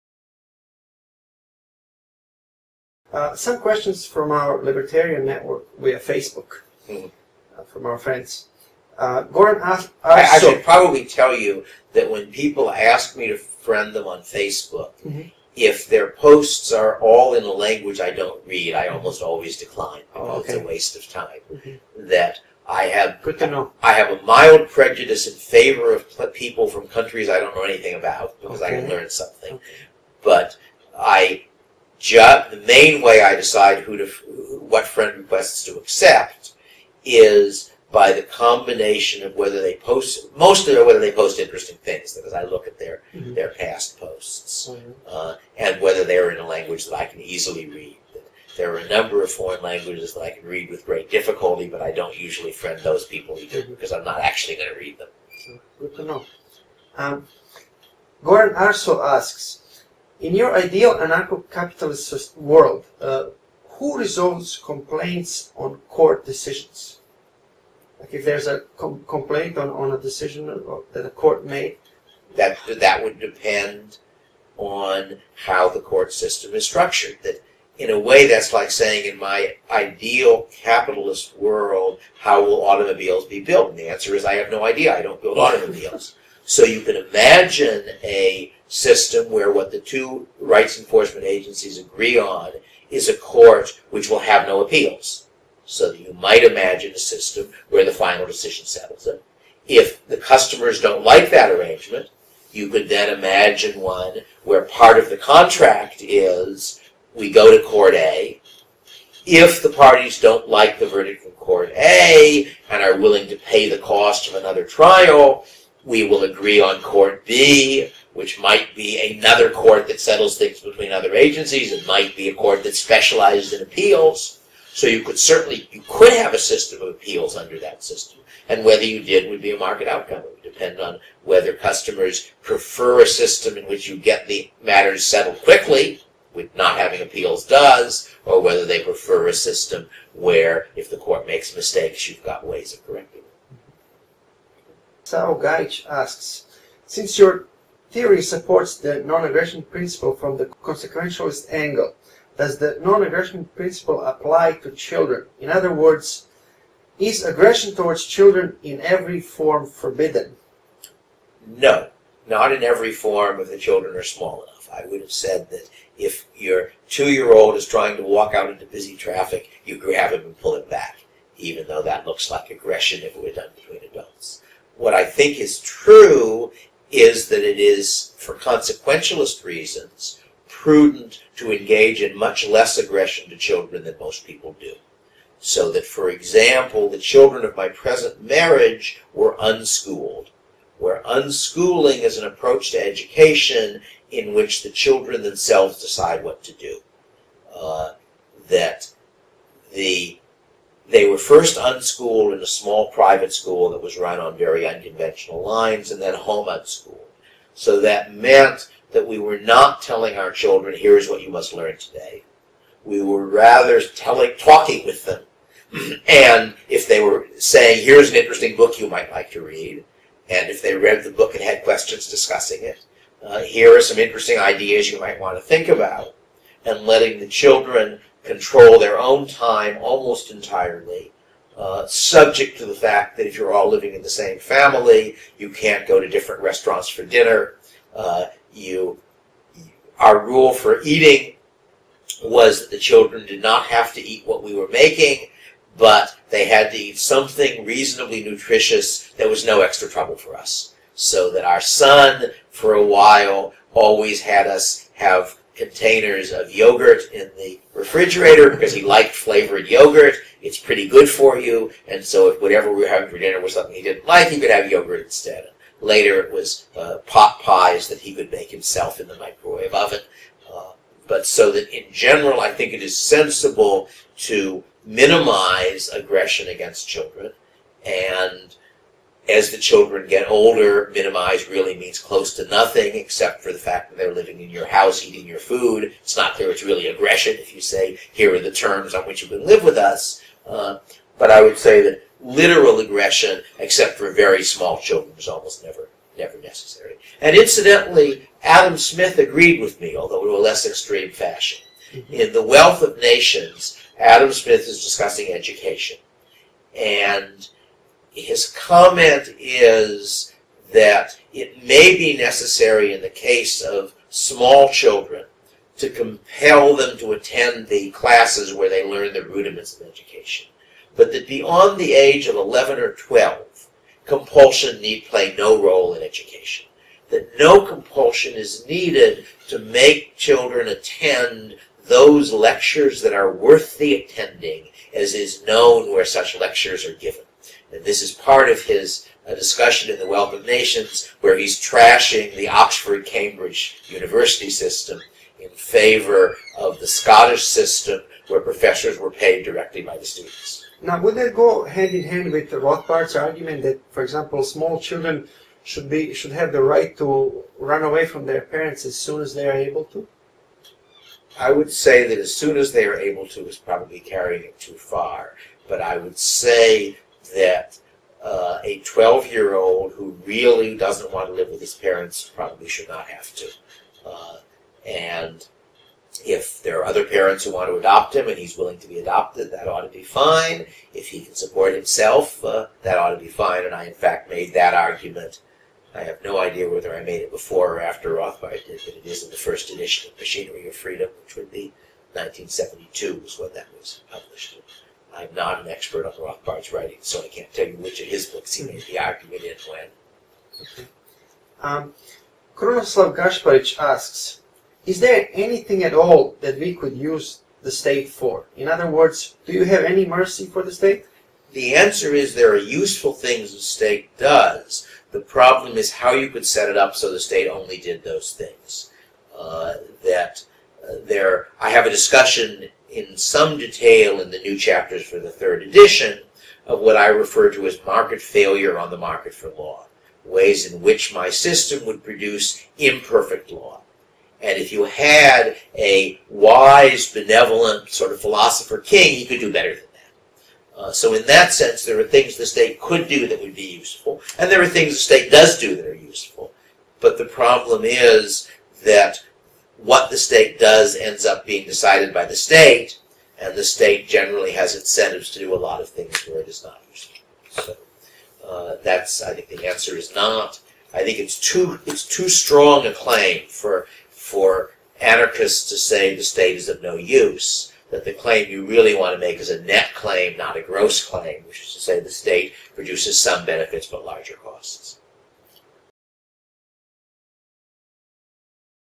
A Conversation With David Friedman - part 6: Court Complaints, NAP&Children, Role for State
Conversation with Dr. David Friedman about some of the solutions and potential issues of anarcho-capitalism, as well as possible ways of advancing ideas of free society and turning them into reality.  Part 6: Resolving Court Complaints, Non-aggression & Children, & Zero Role for the State.

Interview by Jadranko Brkic, Managing Director at Freedom and Prosperity TV: libertarian network of alternative media in Western Balkans.  Hong Kong, May 21st, 2014.

Naslov: Razgovor sa Davidom Friedmanom, 6. dio: Rješavanje sudskih žalbi, neagresija prema djeci, i nulta uloga za državu

Razgovor sa doktorom Davidom Friedmanom o nekim od rješenja njegove verzije anarho-kapitalizma, o potencijalnim problemima anarho-kapitalizma, kao i mogućim načinima ostvarenja slobodnog društva.  Šesti dio: Rješavanje sudskih žalbi, neagresija prema djeci, i nulta uloga za državu.

Intervjuisao Jadranko Brkić, upravni direktor iz organizacije Sloboda i Prosperitet TV: libertarijanska mreža alternativnih medija na zapadnom Balkanu.  Hong Kong, May 21st, 2014.  Saznajte više na našem vebsajtu na adresi